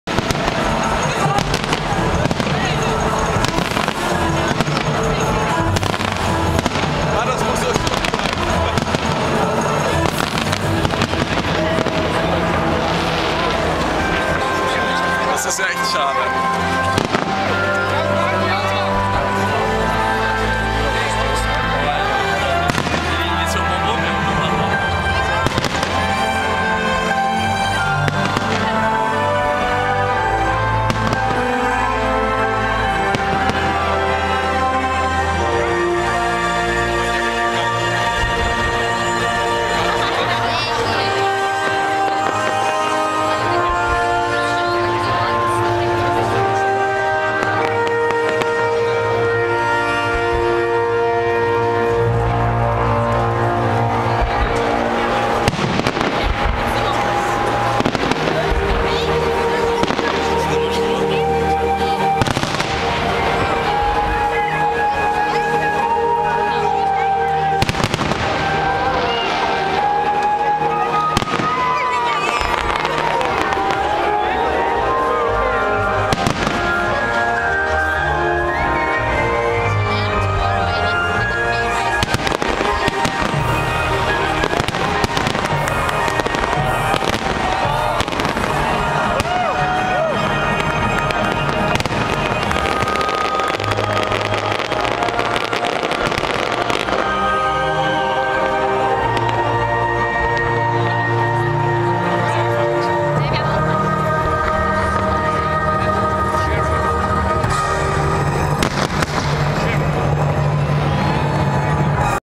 BERLIN HISTORY- 09 November 2009..AVI
BERLIN- Der Mauerfall am 9. November 1989.  "20 Jahre friedliche Revolution".  In the video: CLÉDIO OLEQUES and HENNING in 09 of November 2009- "Branderburg Gate"- Berlin City.